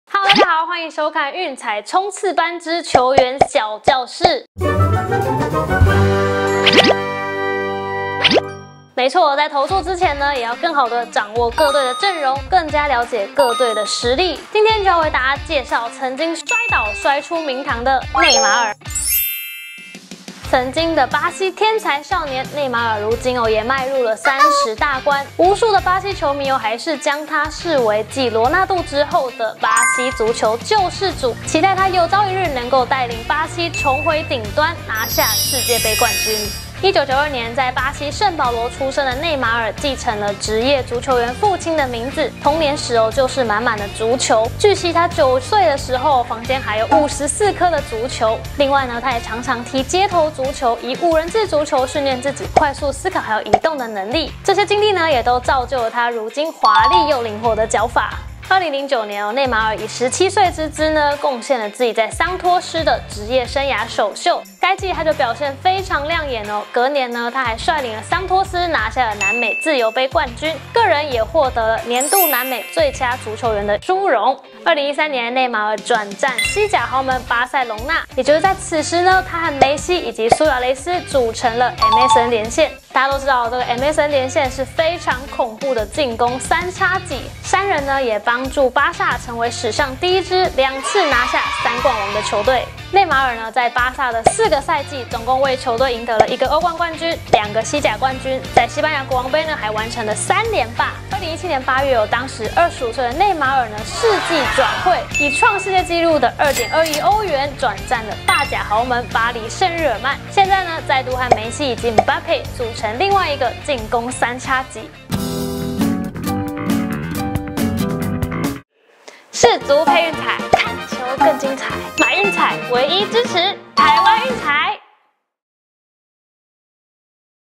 世足球星小教室 世界盃壓力太沈重 內馬爾力拼率「森巴軍團」重返榮耀（上）
✨《Go Goal 勁球網》世足專區上線囉✨

世足賽倒數18天，《Go Goal 勁球網》將會每天提供您不同的球星介紹，讓您可以更加掌握各隊陣容!!!

曾經的巴西天才少年，內馬爾(Neymar)如今也踏入30大關，無數巴西球迷都將內馬爾視為繼羅納度(Ronaldo)後，巴西足球的救世主，但內馬爾始終無法將巴西帶回世界頂端，如今年滿30歲的他，將帶著更成熟的球技，率領巴西在世界盃上過關斬將。

📝新聞看這裡：
世界盃壓力太沈重　內馬爾要率「森巴軍團」重返榮耀

－我是分隔線－《Go Goal 勁球網》追追追～追起來－